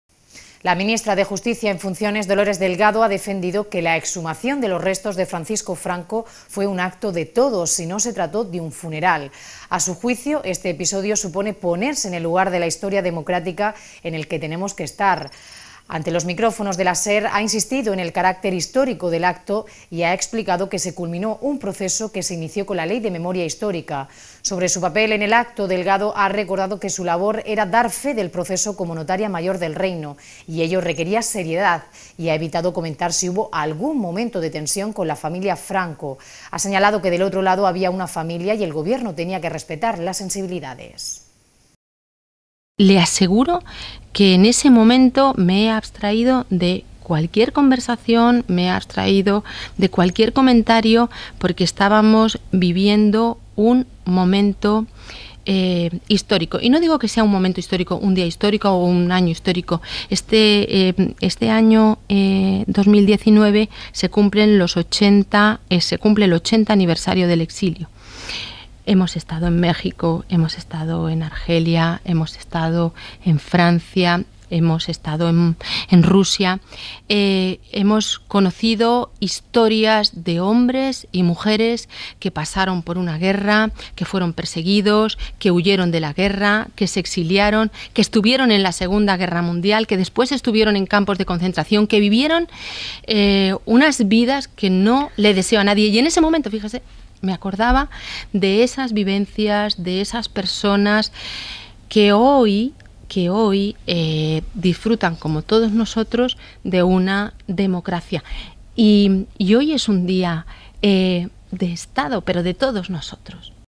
Noticia - La ministra de Justicia en funciones, Dolores Delgado, sobre la exhumación de Franco
La ministra de Justicia en funciones, Dolores Delgado, ha defendido que la exhumación de los restos del dictador Francisco Franco de este jueves fue un "acto de todos" y no se trató de un funeral. A su juicio, este episodio supone "ponerse en el lugar de la historia democrática en el que tenemos que estar". Ha insistido en el carácter "histórico" del acto de ayer y ha explicado que se culminó un proceso que se inició con la Ley de Memoria Histórica. Sobre su papel en el acto, Delgado ha recordado que su labor era dar fe del proceso como notaria mayor del Reino y ello "requería seriedad". Y ha evitado comentar si hubo algún momento de tensión con la familia Franco, ha señalado que del otro lado "había una familia" y el Gobierno tenía que respetar "las sensibilidades".

English:

Acting Justice Minister Dolores Delgado has argued that the exhumation of the remains of the dictator Francisco Franco this Thursday was an "act of all" and was not a funeral. In his opinion, this episode involves "putting oneself in the place of democratic history in which we have to be." He has insisted on the "historical" character of yesterday's act and explained that a process that began with the Law of Historical Memory was completed. On his role in the act, Delgado recalled that his job was to attest to the process as a notary major of the Kingdom and that "required seriousness." And he has avoided commenting if there was any moment of tension with the Franco family, he said that on the other side "there was a family" and the Government had to respect "sensibilities."

Live streaming